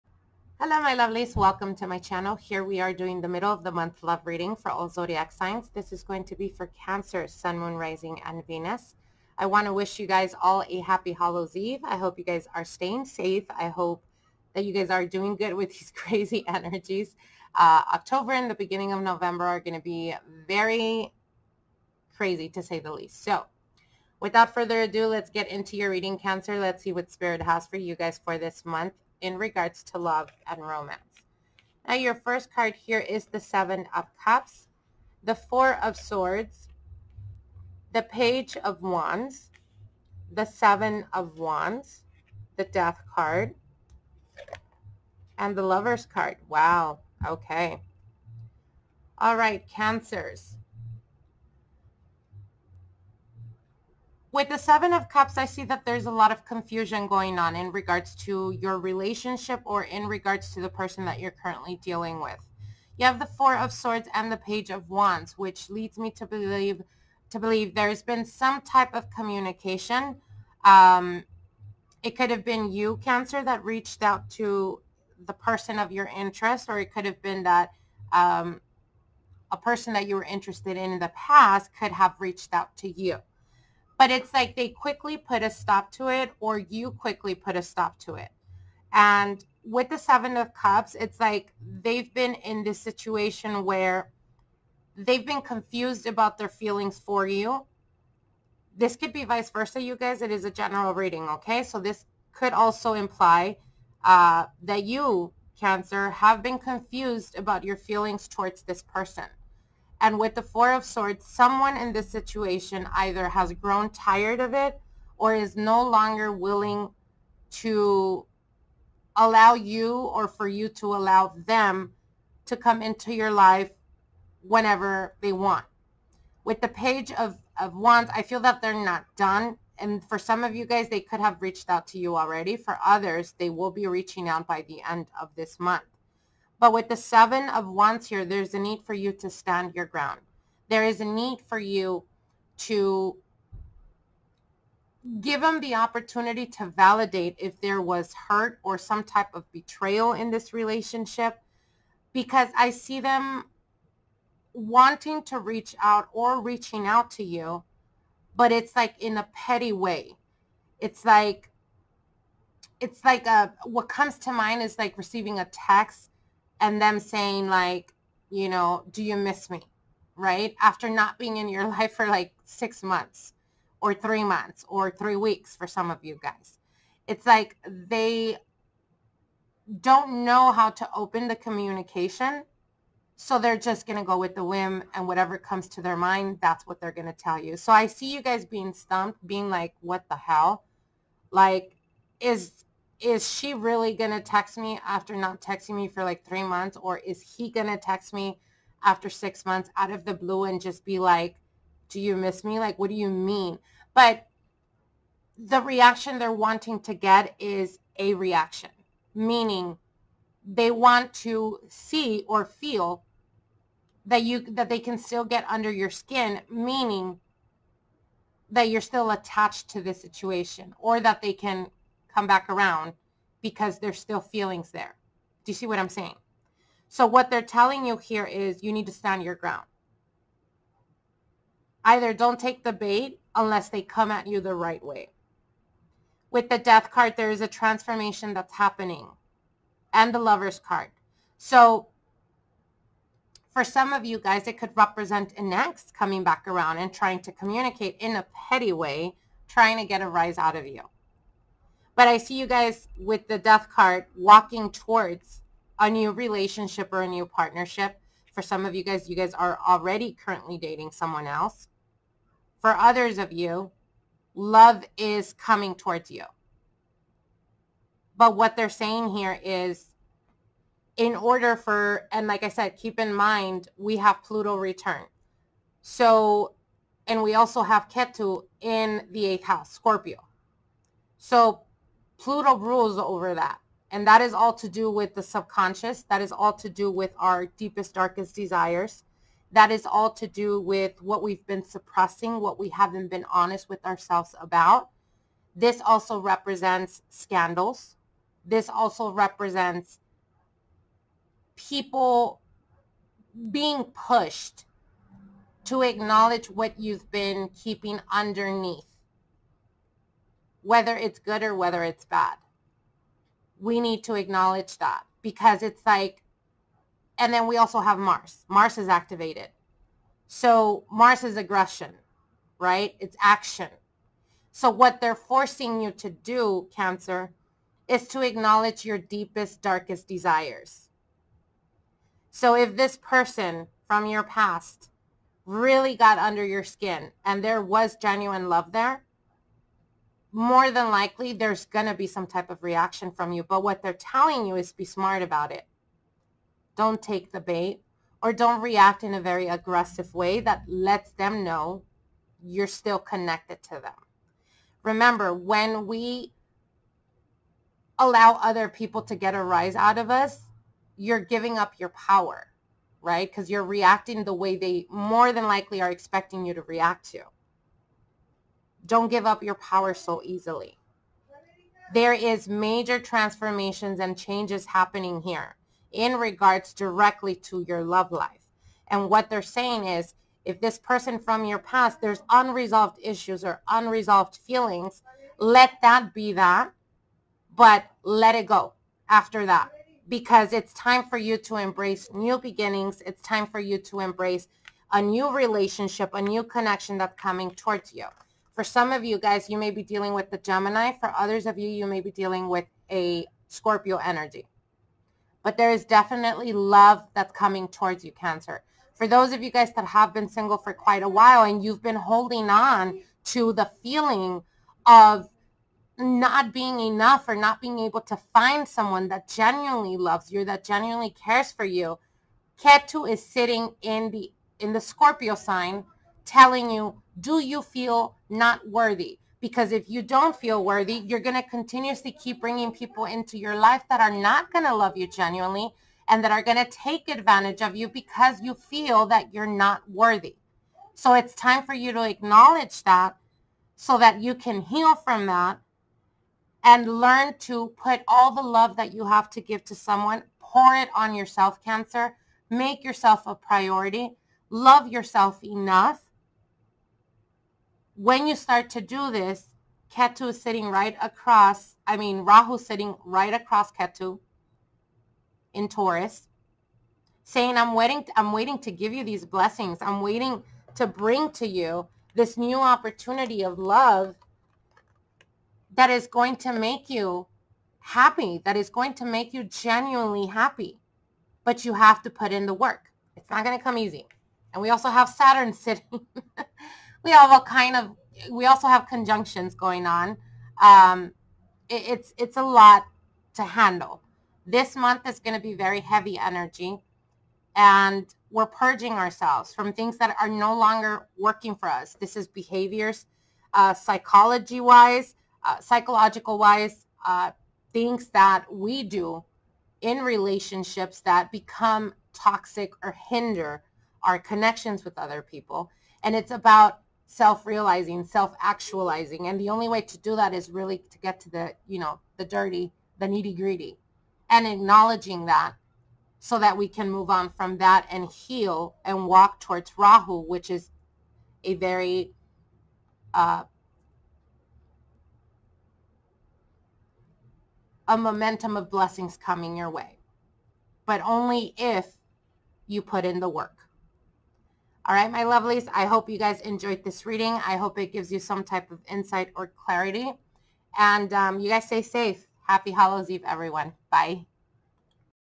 CANCER OCTOBER LOVE 2020 "SOMEONE WANTS YOU BACK"
Hello my lovelies here is the middle of the month October 2020 Love readings for all signs hope it gives you clarity and insight in regards to love and romance hope you guys enjoy and Happy Hollow's Eve!

3 Card Reading
30 Min Reading
1 Hr Reading